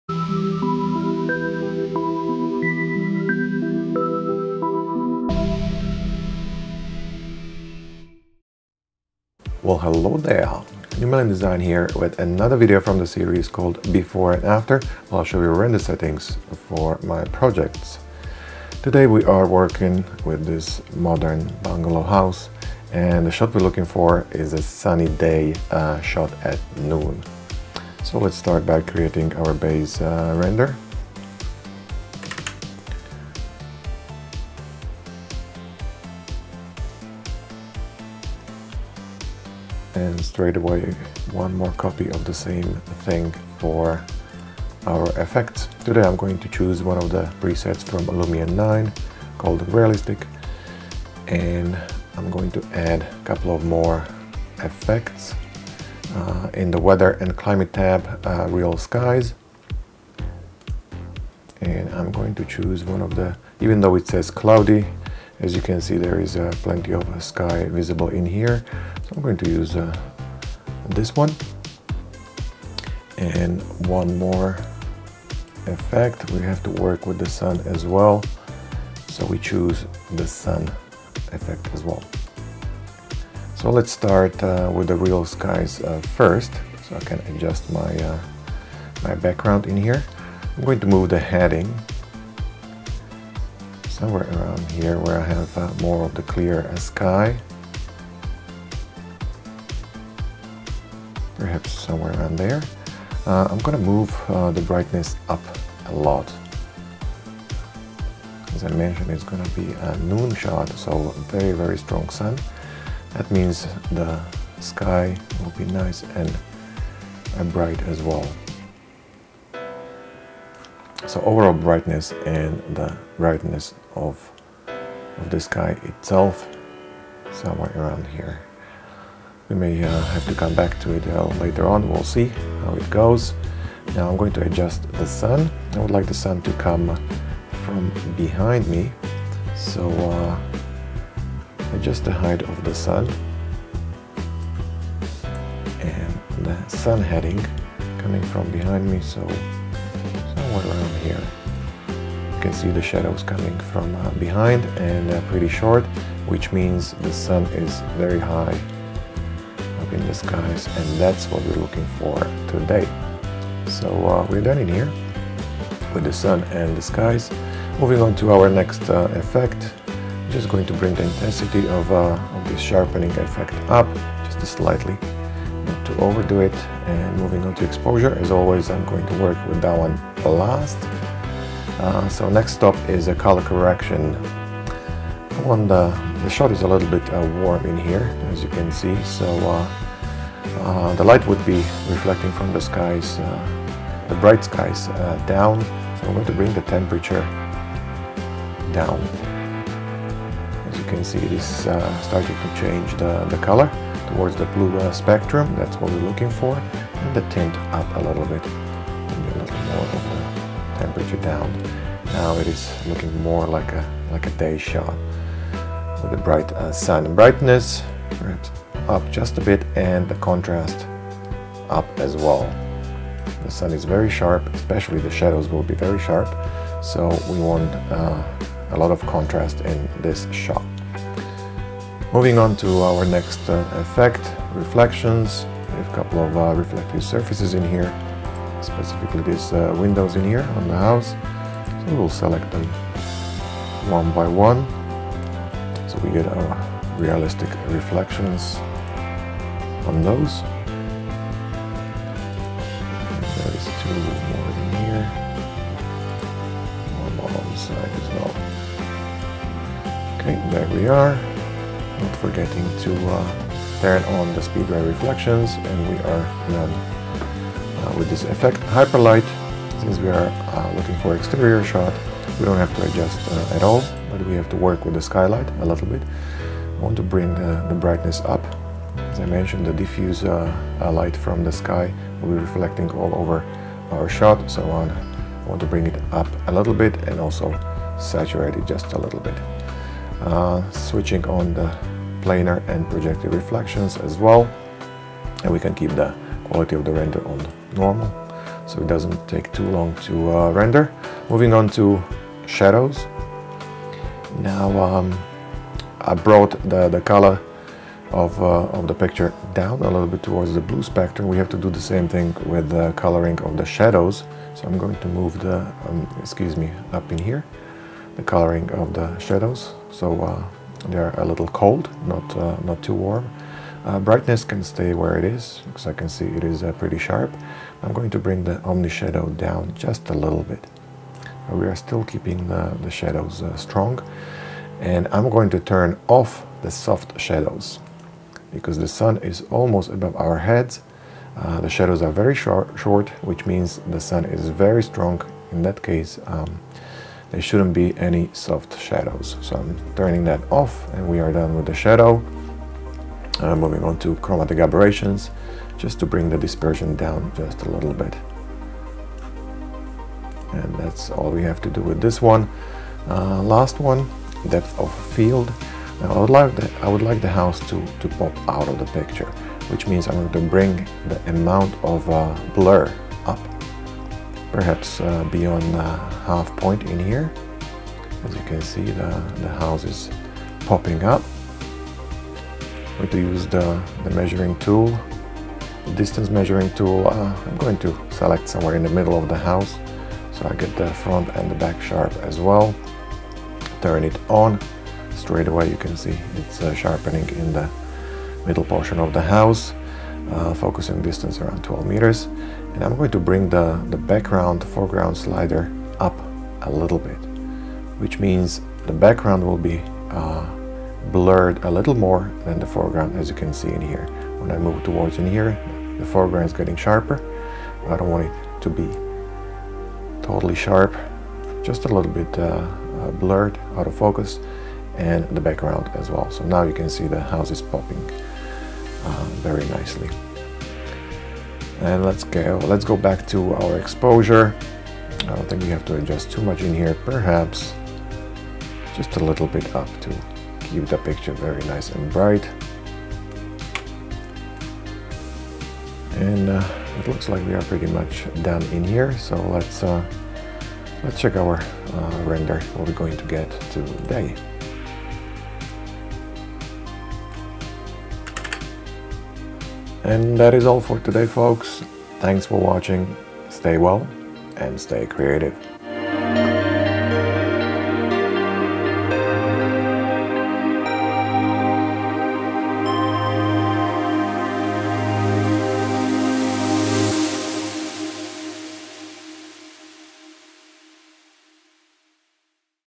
Lumion 9 Modern Bungalow Render Settings Tutorial
100% Lumion 9.5 Pro Render Settings Tutorial for Before and After shots of this Modern Bungalow house. No Post production.